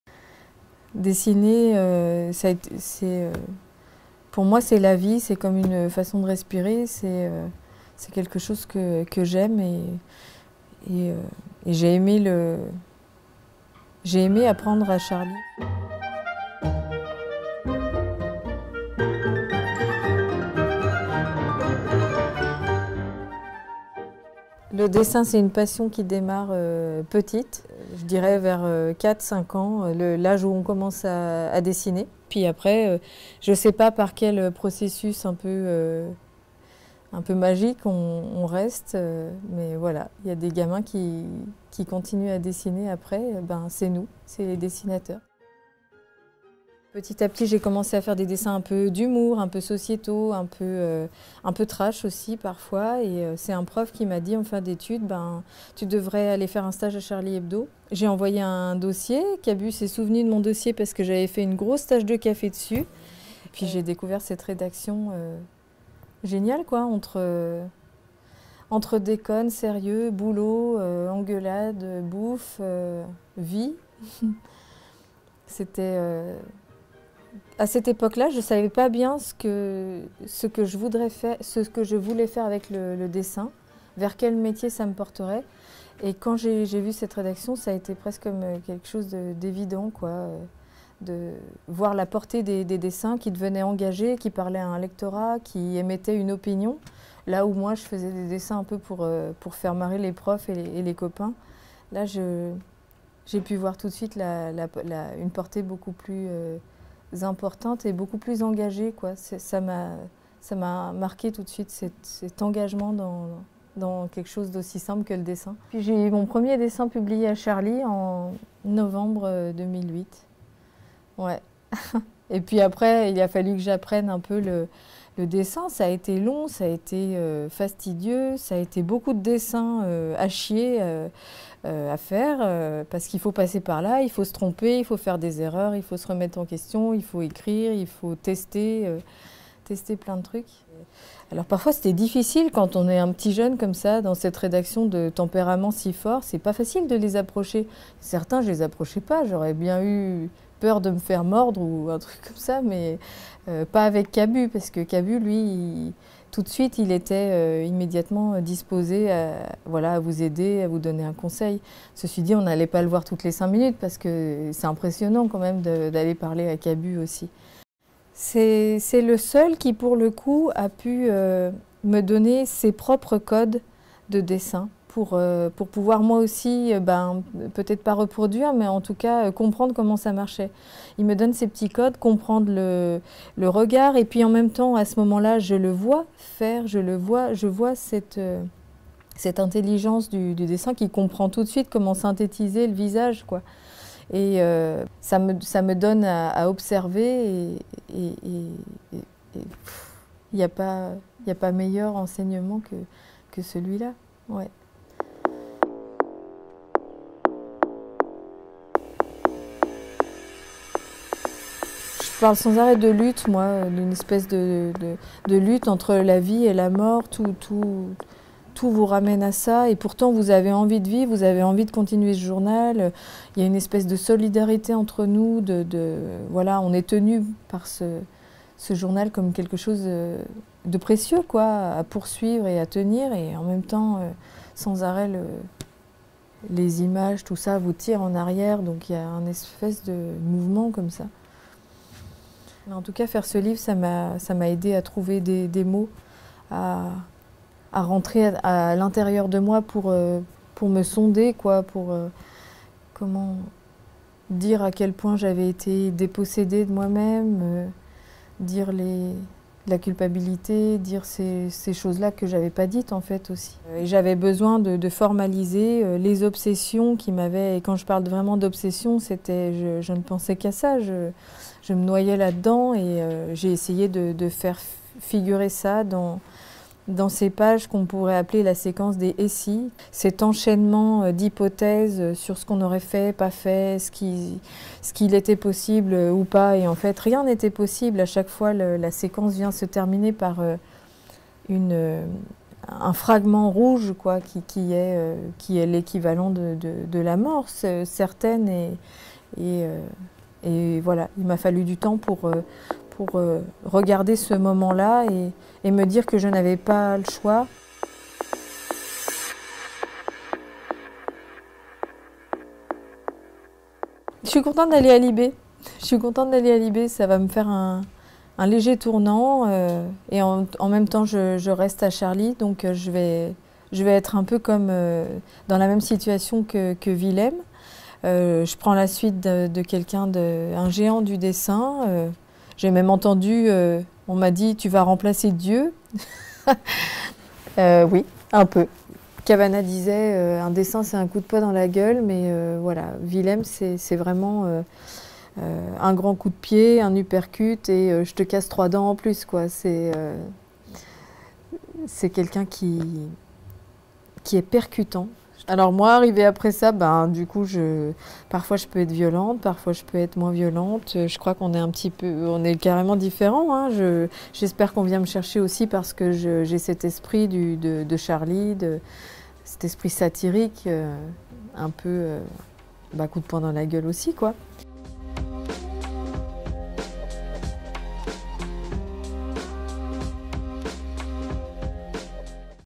Coco: "Il faut se tromper, faire des erreurs, tester plein de trucs" | AFP
Rescapée de l'attentat qui a décimé la rédaction de Charlie Hebdo, son école, la caricaturiste Coco retrace dans "Dessiner encore" son combat vers la reconstruction, affûtant ses crayons avant de prendre, à 38 ans, la succession de Willem à Libération